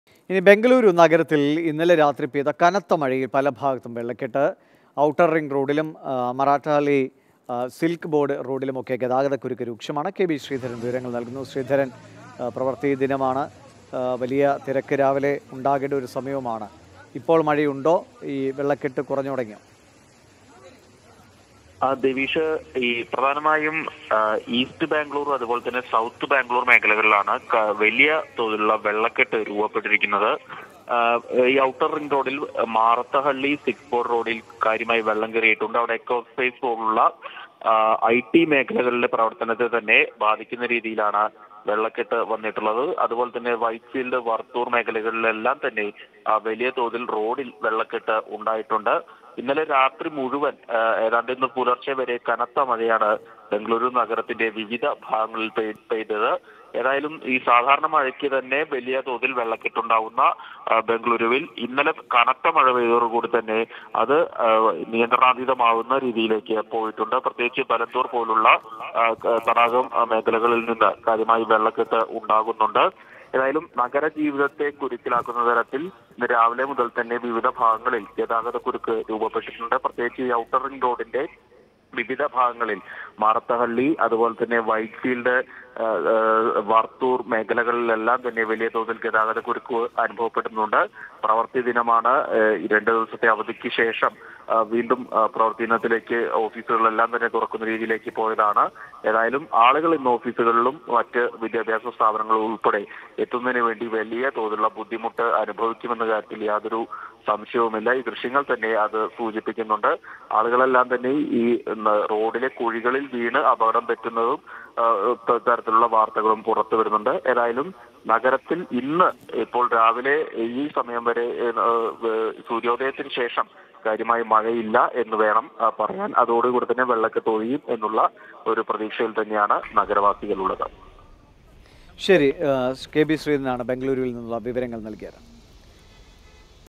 ബെം​ഗളൂരു ന​ഗരത്തിൽ കനത്ത മഴ; പലഭാ​ഗത്തും വെള്ളക്കെട്ട് | Bengaluru Rain | Mathrubhumi News
ബംഗളൂരു നഗരത്തിൽ ഇന്നലെരാത്രി പെയ്ത കനത്ത മഴയിൽ പലഭാഗത്തും വെള്ളക്കെട്ട്. ഔട്ടർ റിംഗ് റോഡിലും മാറത്തഹള്ളി-സിൽക്ക് ബോർഡ് റോഡിലും ഗതാഗതക്കുരുക്ക്.

Happy viewing!